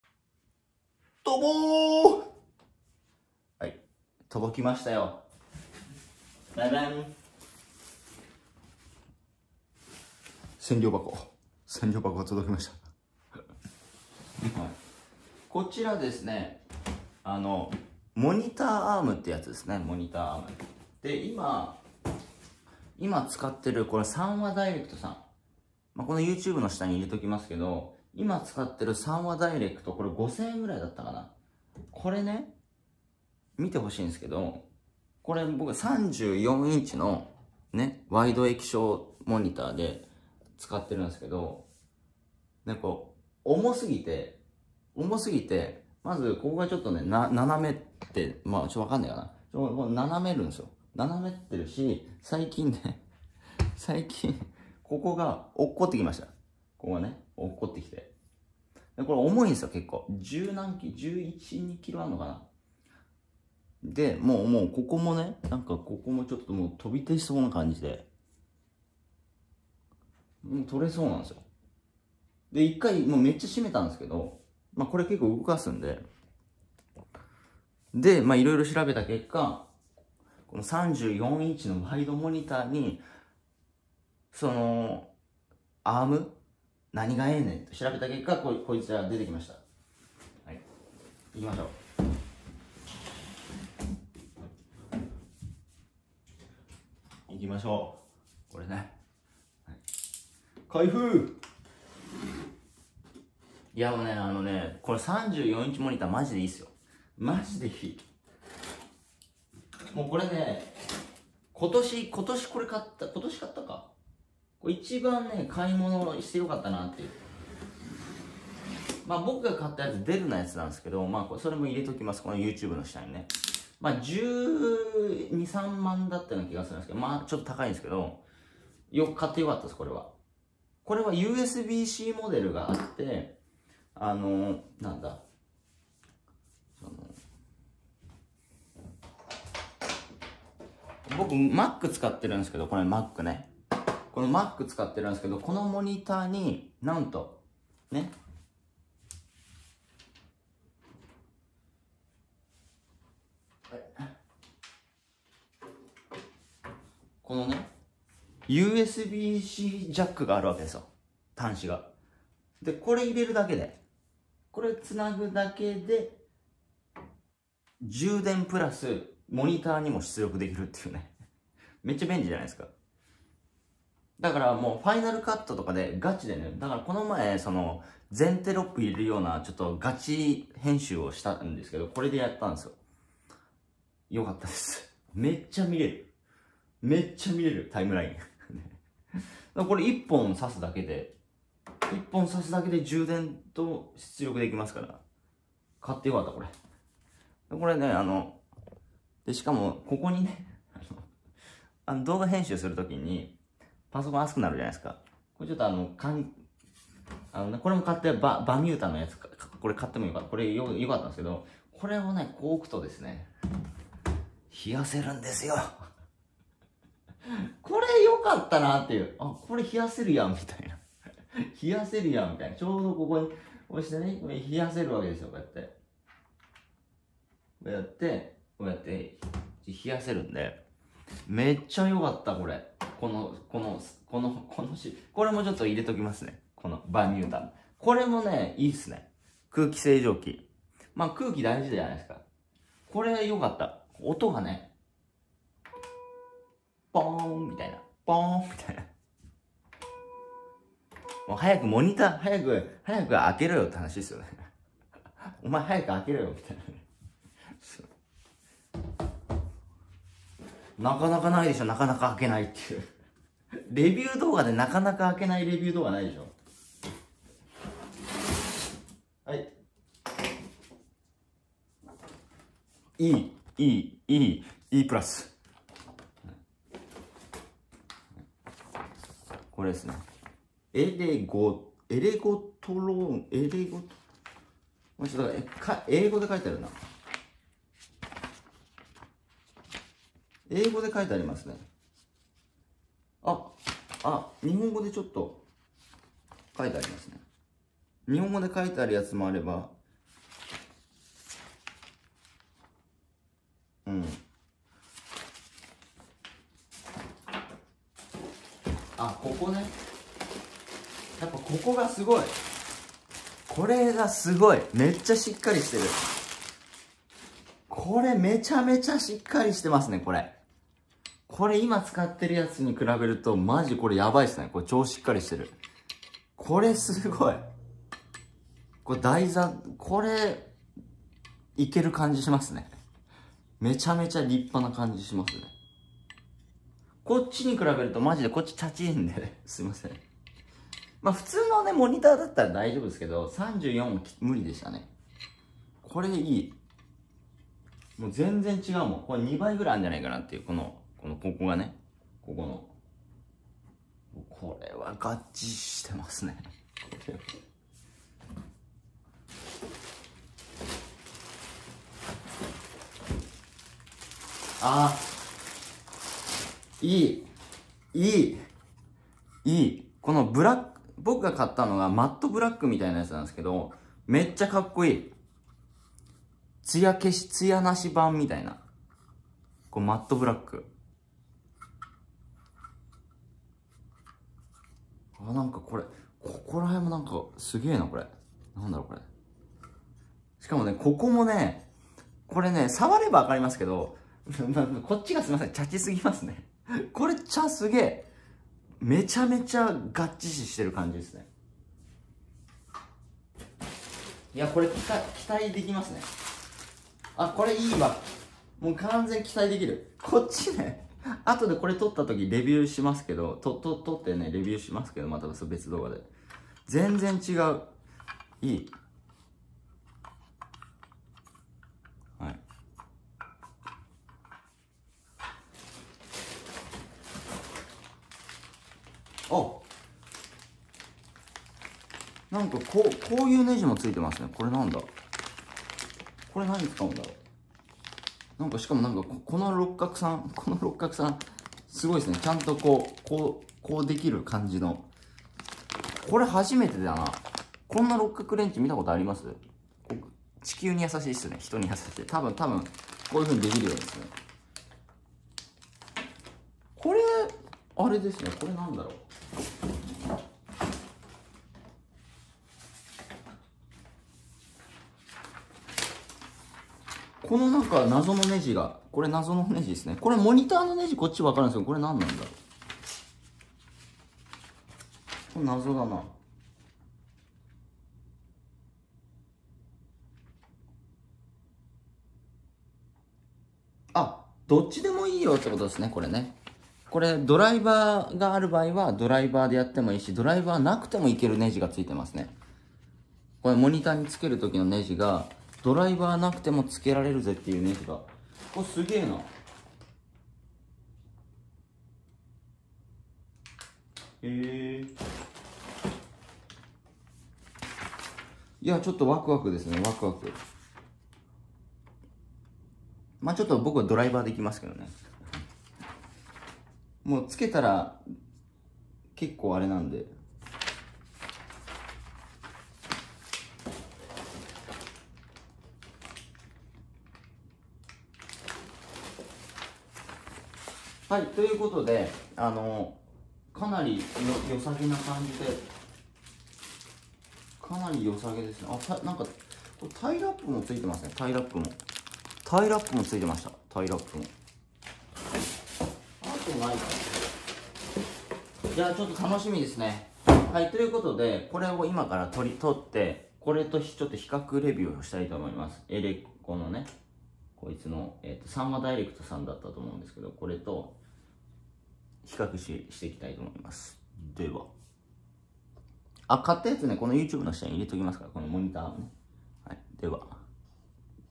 34インチワイドモニター用にエルゴトロン LX デスクマウント モニターアームを購入しました！
購入したモニターアーム⇒
使ってるモニター⇒
使ってるキーボード⇒
使ってるマウス⇒
使っているマイク⇒
使ってる空気清浄機⇒